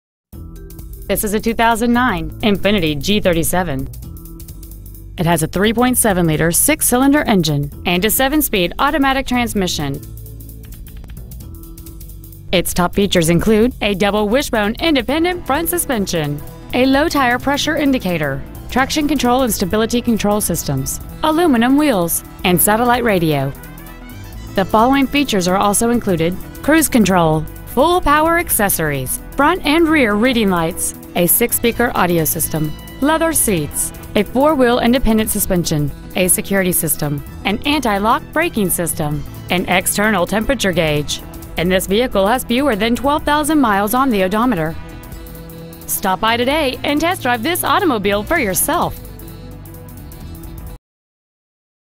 2009 INFINITI G37 Abilene TX
Year : 2009

 Make : INFINITI

 Model : G37

 Engine : 3.7L V6 24V MPFI DOHC

 Trans . : 7-SPEED AUTOMATIC

 Exterior : BLACK OBSIDIAN

 Miles : 11,194

 SouthWest Ford

 800-NEW-FORD

 3000 Ft Worth Hwy

 2009 INFINITI G37  TX